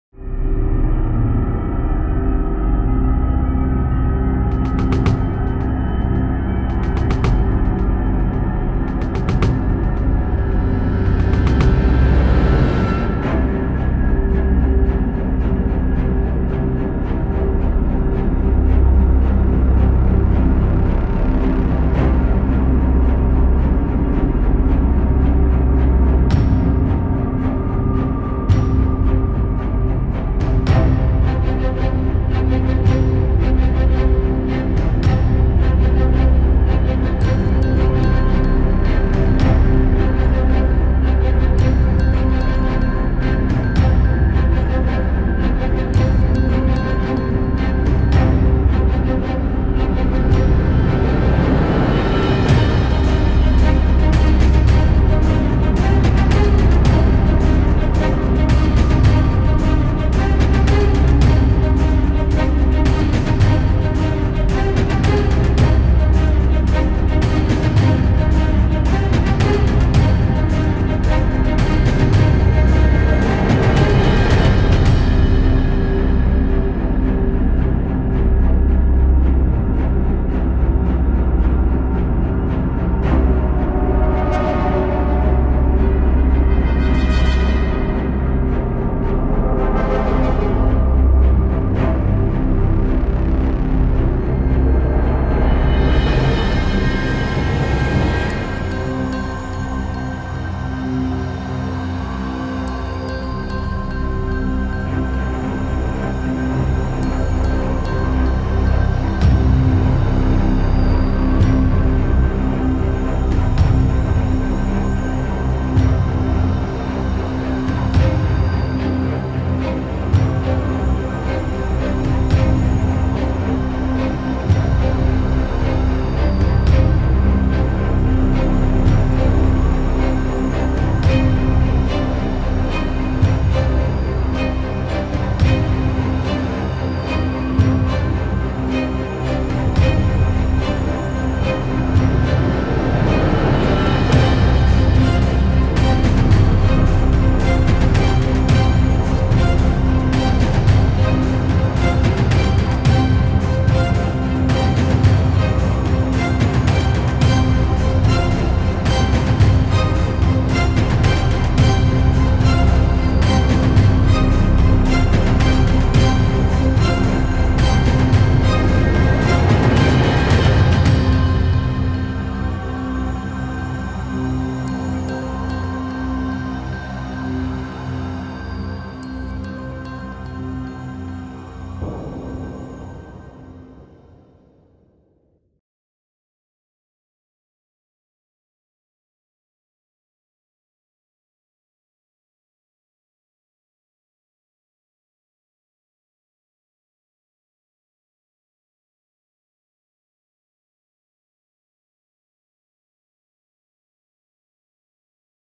Symphony Of Sands: Defending Pyramids From Alien Onslaught
👽 The desert sands hold secrets, and when combined with an otherworldly invasion, it creates an exciting narrative. The word “melody” adds a musical touch.

Imagine beings defending the pyramids through harmonious melodies. This invokes a sense of wonder and protection.

Dare to explore the darkness with us... if you dare. 💀